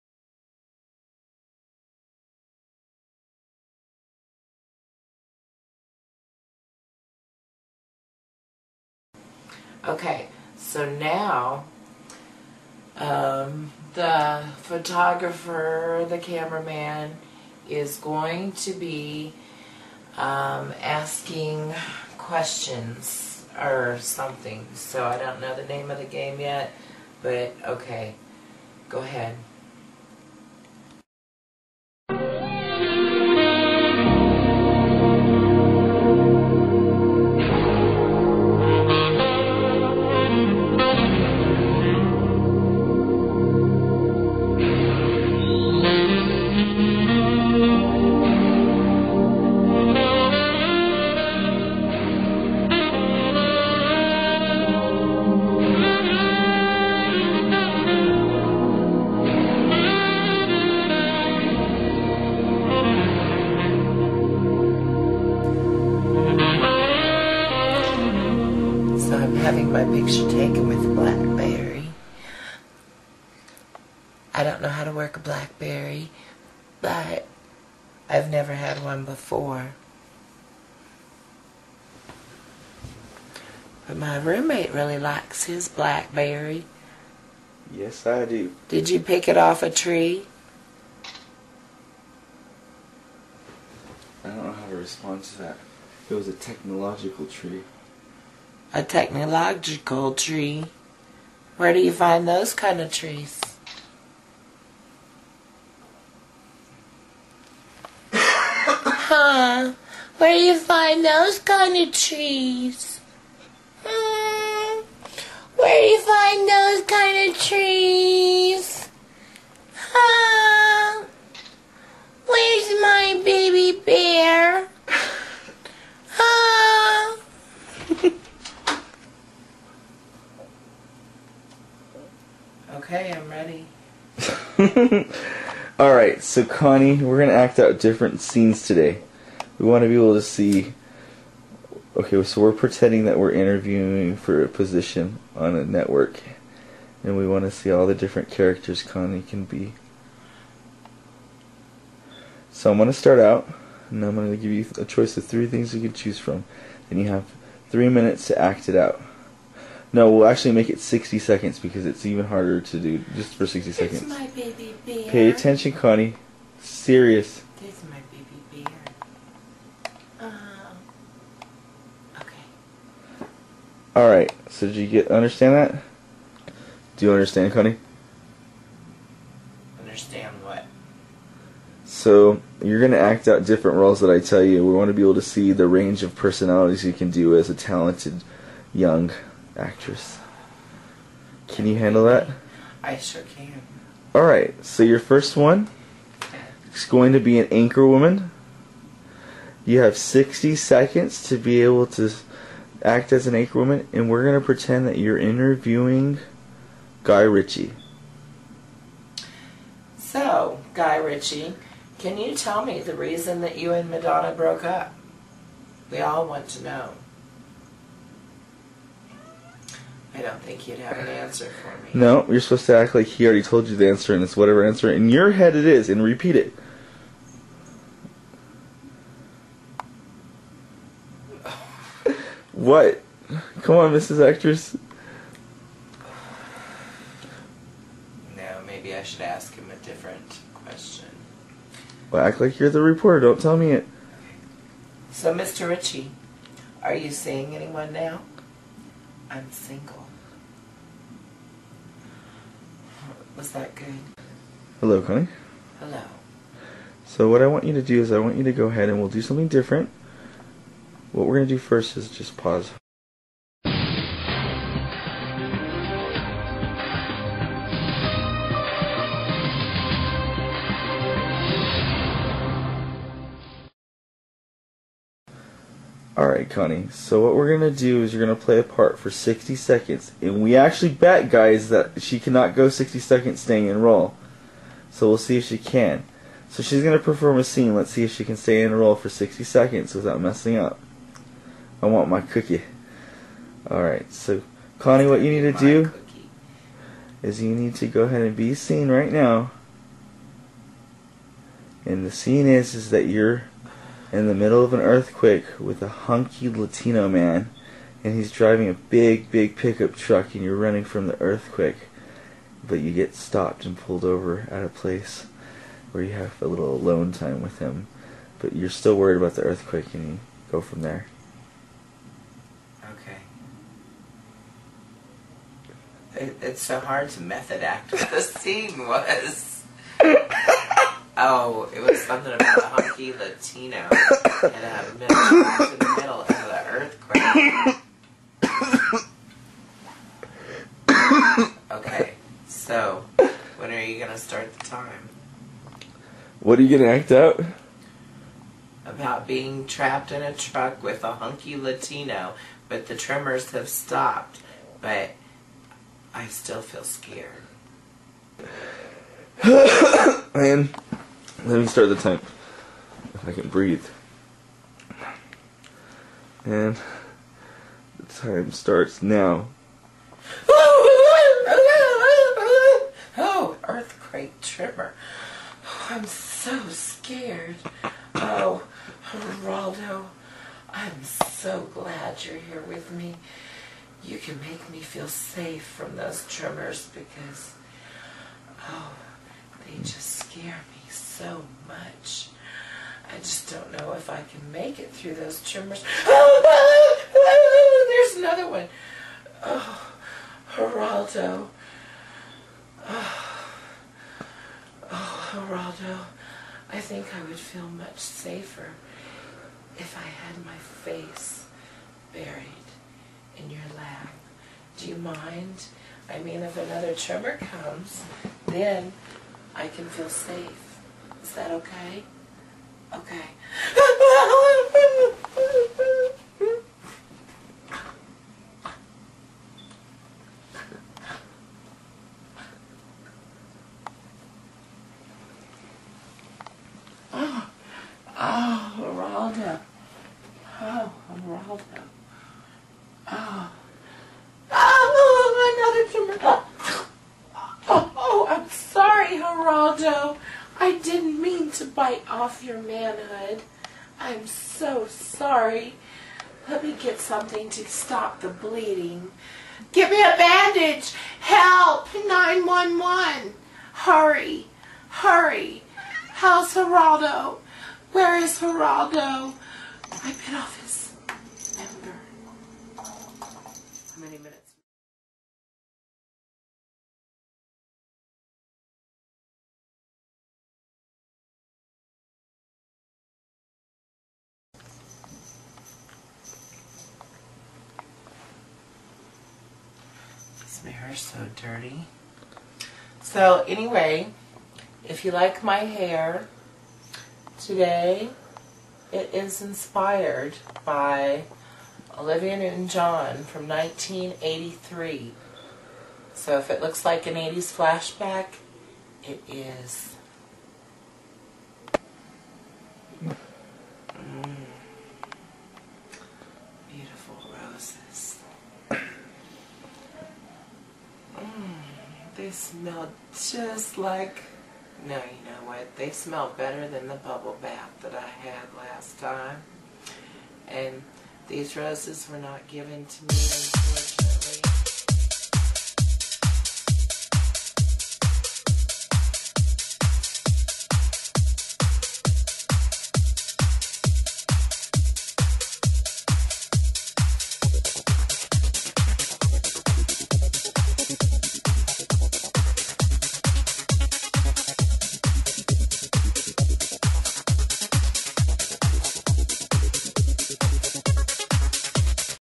Method Acting
the cameraman wants Connie to portray her skills as an actress... he's
gave her a couple of assignments, but will she understand them? And
will she win the bet that she can stay in character for over 60
seconds? Will our leading lady win an Oscar for her performance? Or
will her talents be more classified for a porno sensation? Watch to see
the hilarious results, doing method acting as only Connie can!